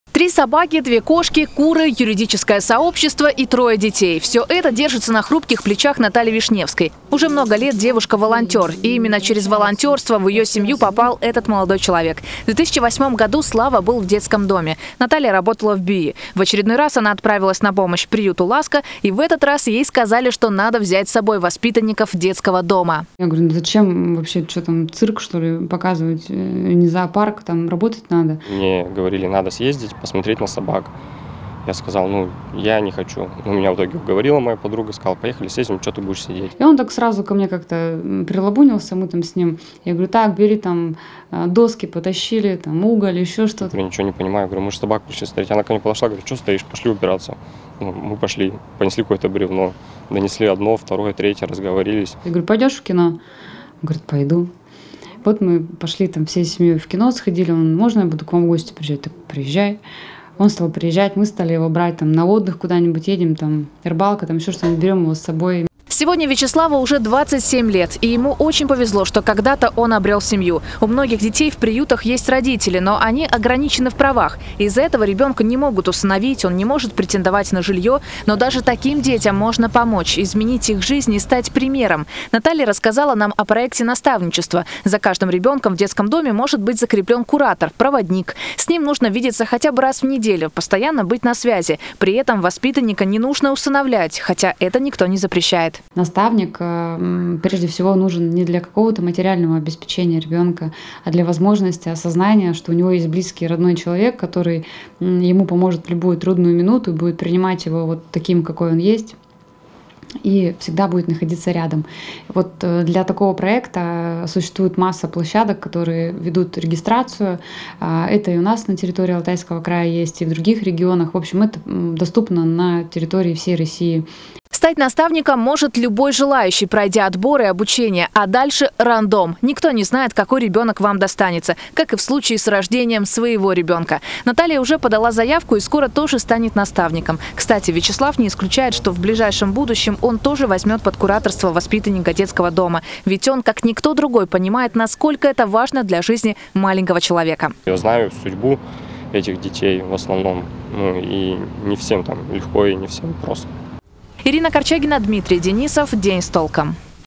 "Быть примером". Стать наставником для подростка из детского дома может каждый
Наш сюжет об уникальном проекте. Его суть в том, что пройдя обучение, любой желающий может помочь ребёнку, оставшемуся без родителей, не потеряться в этом мире. Направить, дать совет и быть на связи в трудную минуту. Подробнее в сюжете.